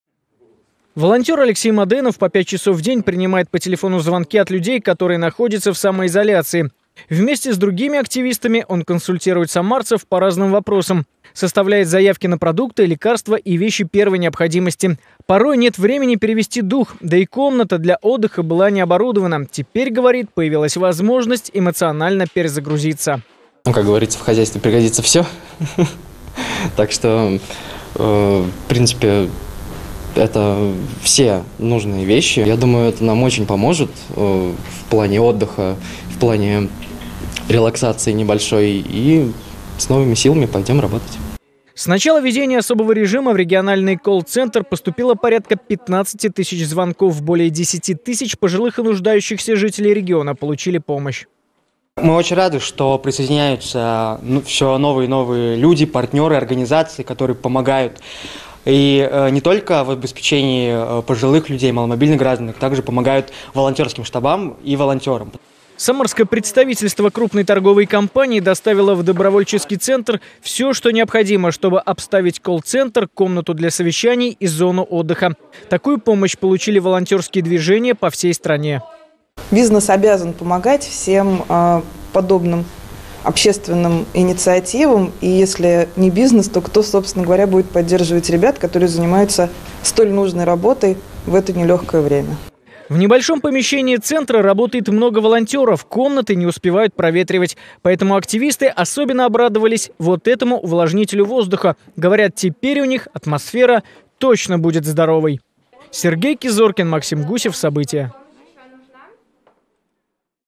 Самарским волонтерам стало работать комфортнее
Как изменился интерьер комнаты отдыха и коллцентра увидел Сергей Кизоркин.